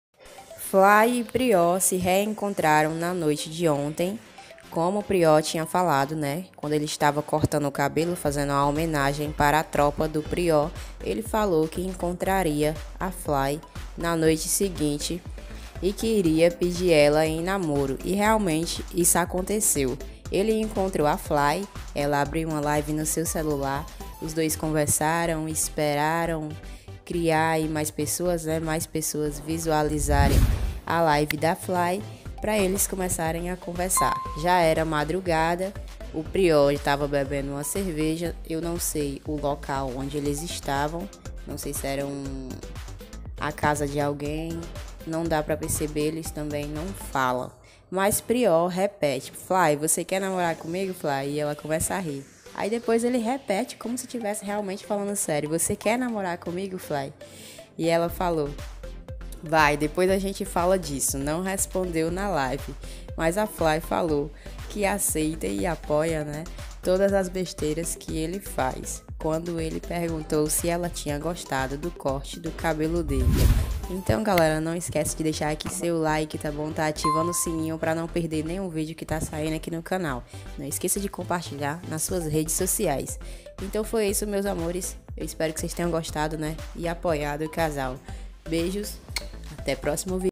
Felipe prió e Flayslane estão namorando ? 🔥❤️😍
Felipe prió e Flaylane estão namorando ?
Na noite de ontem eles se encontraram, como pior tinha citado em sua live. Já era madrugada quando eles abriram uma live no celular da flay,
E prió perguntou flay você quer namorar comigo ?
Ela riu e ele retornou a perguntar 🔥😁❤️
E aí vocês apoia esse casal

BBB

Prió pediu flay em namoro